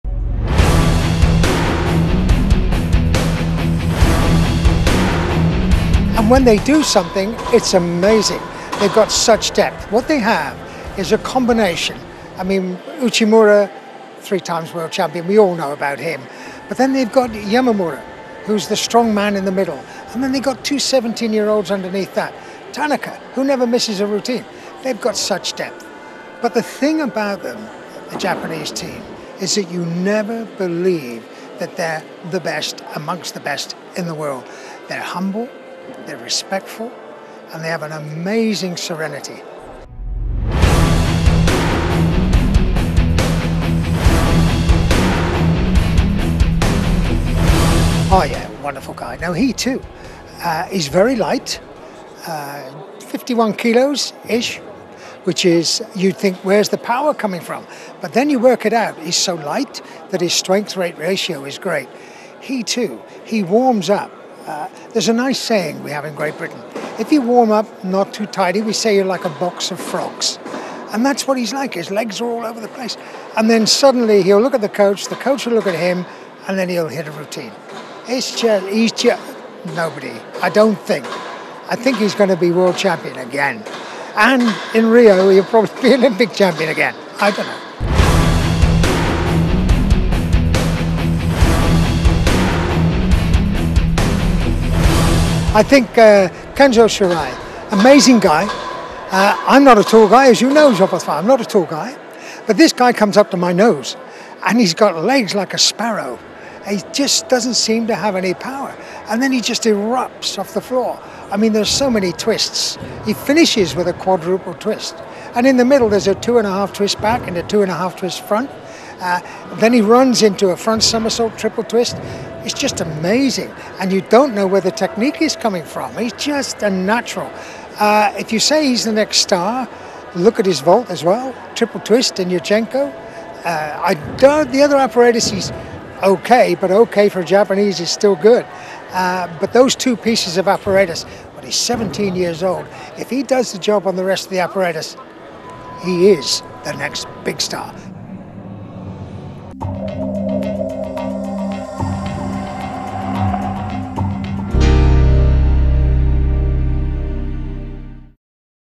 Powerhouse Japan - 2013 Artistic Worlds
FIG Official -- 2013 Artistic Worlds, Antwerp (BEL) - With Kohei Uchimura, Ryohei Kato, Kenzo Shirai, Kazuhito Tanaka, Koji Yamamuro and Kohei Kameyama, Japan Team has moved with talented gymnasts. FIG TV consultant, Mitch Fenner is impressed. Medal Haul is open...